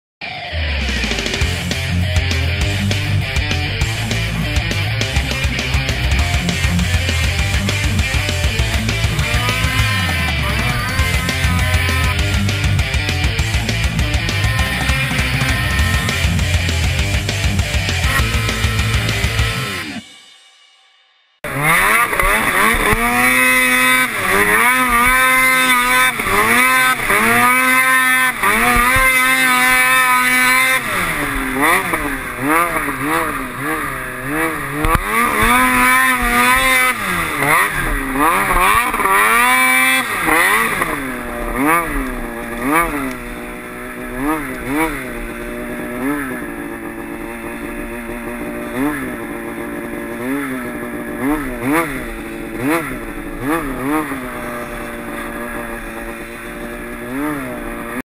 Backcountry Snowmobiling - Hillclimb
Backcountry hydroline snowmobiling on my ski-doo xp rs. Great snow on this day one of the best rides this year!